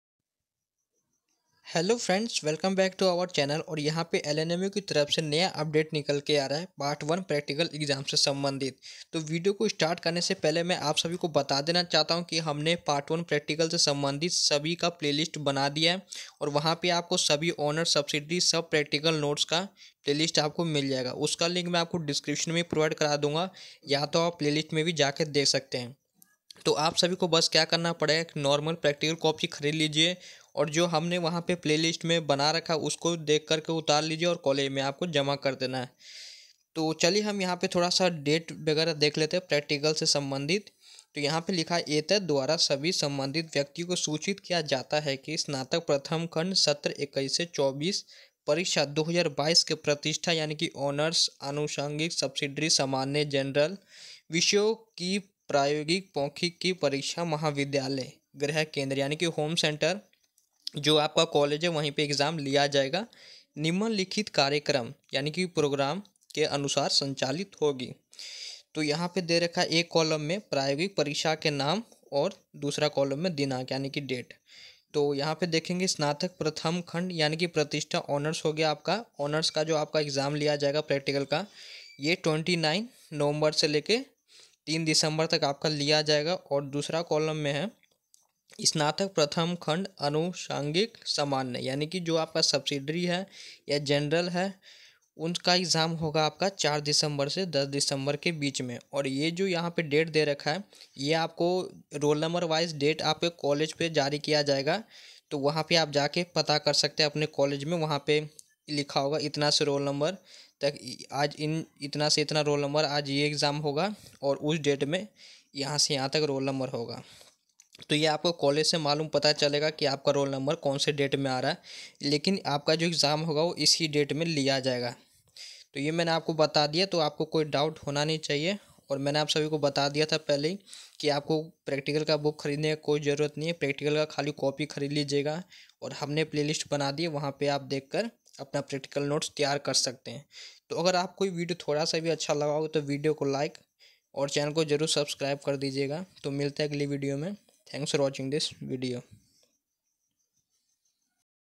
Lnmu part 1 practical exam date 2022
Hello friends !!!

In this video I will tell you about the lnmu part 1 practical exam date.

B.Ed 1st YEAR NOTES